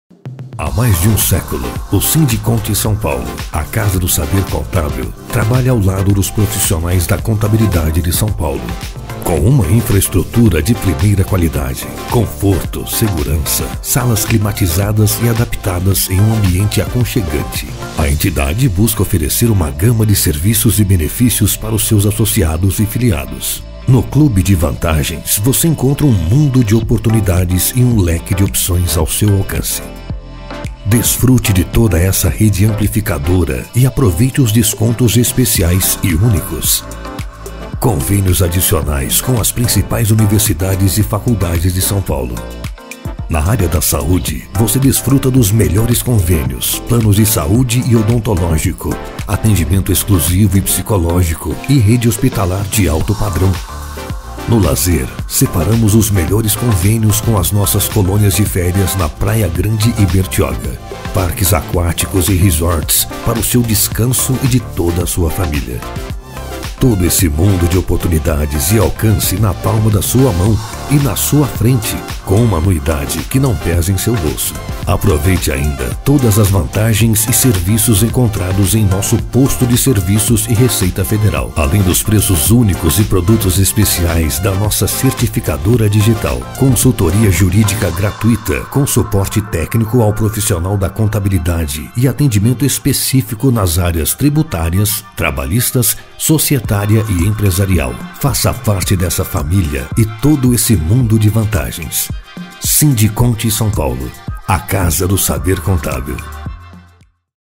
Sindcont-SP 'Casa do Saber Contábil'
Conheça mais sobre o Sindcont-SP e sua gama de atividades, serviços e oportunidades.

Neste vídeo, o profissional da contabilidade e associado à Casa do Saber Contábil conhecerá um pouco mais sobre os benefícios oferecidos.